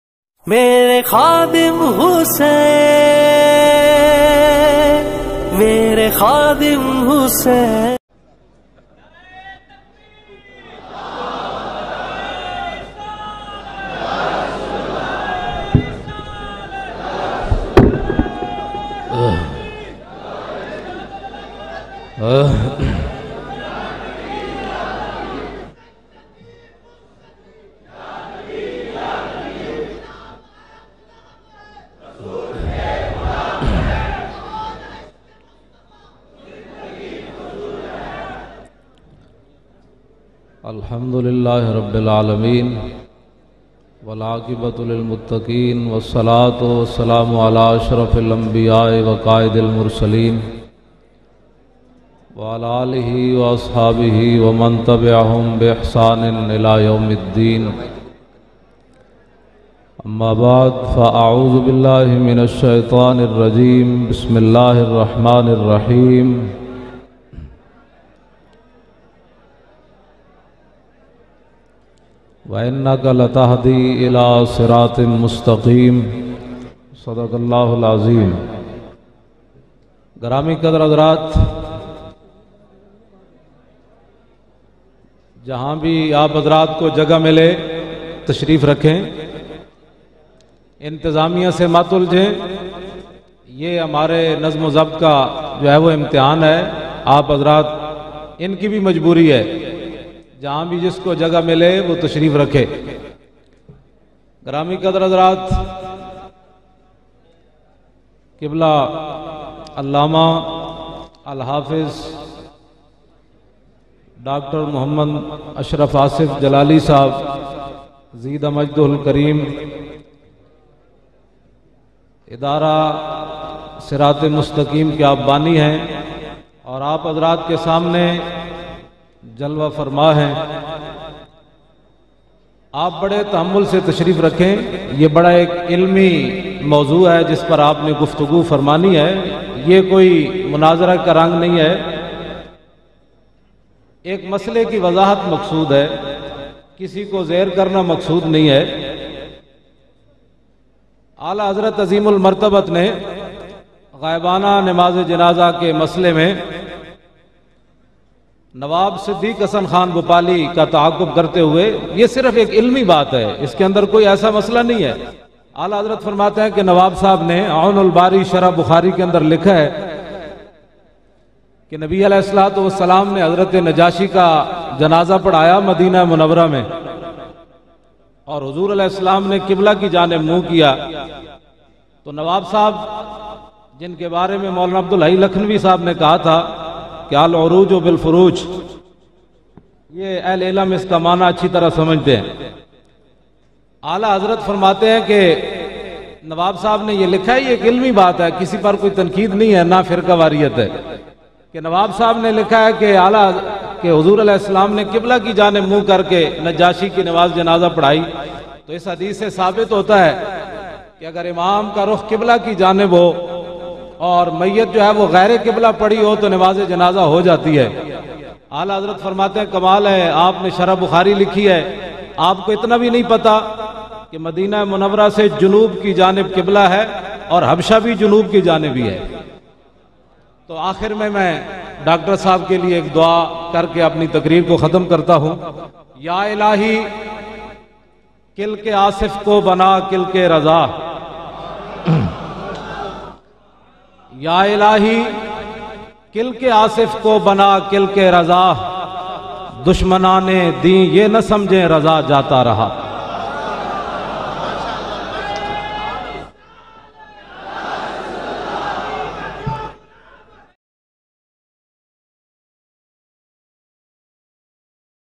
Allama KHADIM HUSSAIN RIZVI || First Bayan of His Life
علامہ حافظ خادم حسین رضوی رحمتہ اللہ تعالیٰ علیہ کی زندگی کا
|| پہلا بیان { سیمینار } ||  غائبانہ نمازِجنازہ

Allama khadim hussain rizvi ki zindgi ka pehla bayan {...Gaibana namaz namaz JANAZA...} ( Seminar )

TWITER LINK